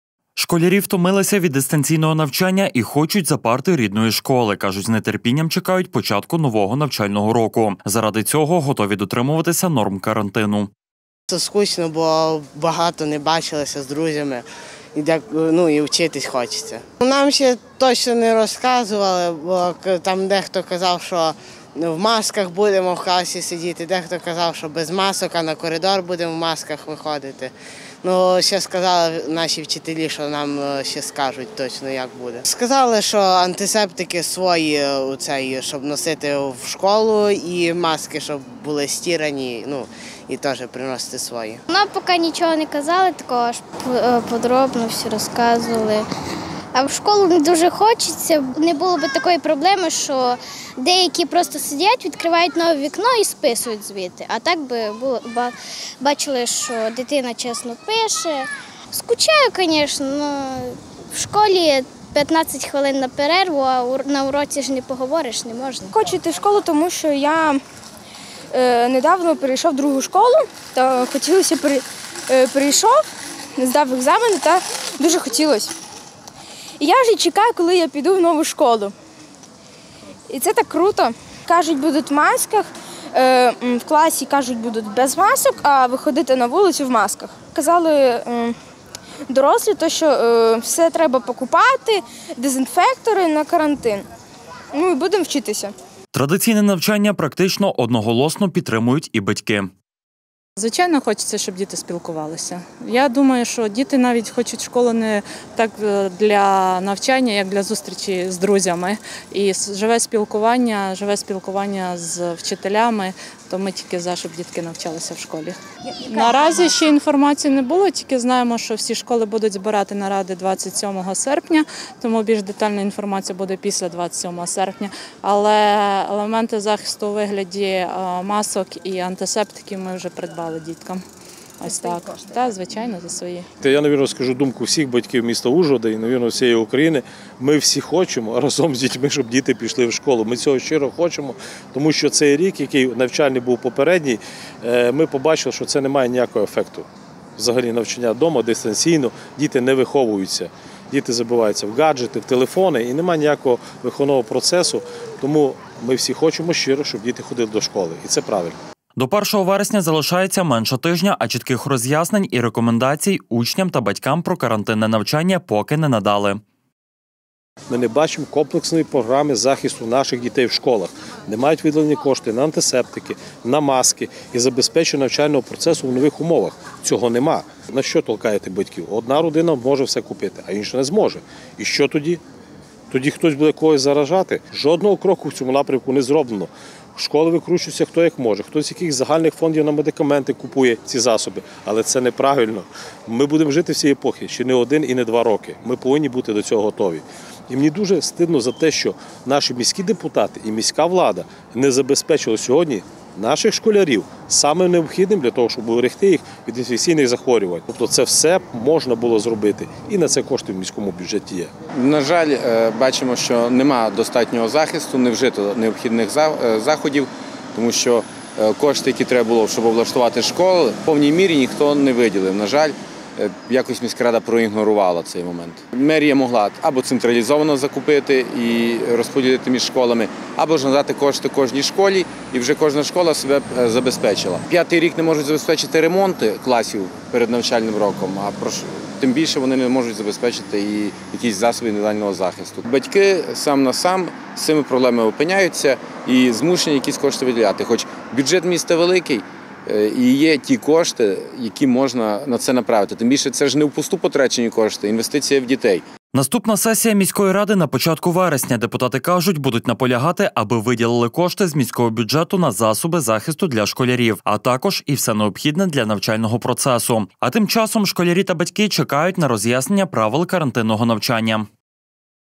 Підготовка до школи
З наближенням 1 вересня школярі та їх батьки активізували підготвку до нового навчального року. Як батьки , так і учні,  практично в унісон, твердять – що хочуть, аби навчання було не дистанційним, а у звичному режимі. Проте епідемія коронавірусу внесла свої корективи  і змусила навчитися  жити  в умовах карантину. Школярі втомилися від дистанційного навчання і хочуть за парти рідної школи! Кажуть, з нетерпінням чекають початку нового навчального року! Заради цього готові дотримуватися норм карантину.Традиційне навчання практично одноголосно підтримують і батьки. До 1 вересня залишається менше тижня, а чітких роз’яснень і рекомендацій учням та їх батькам про карантинне навчання поки не надали. Наступна сесія міської ради – на початку вересня. Депутати кажуть, будуть наполягати, аби виділили кошти з міського бюджету на засоби захисту для школярів,  а також і все необхідне для навчального процесу. А також школярі і  батьки чекають на роз’яснення правил карантинного навчання.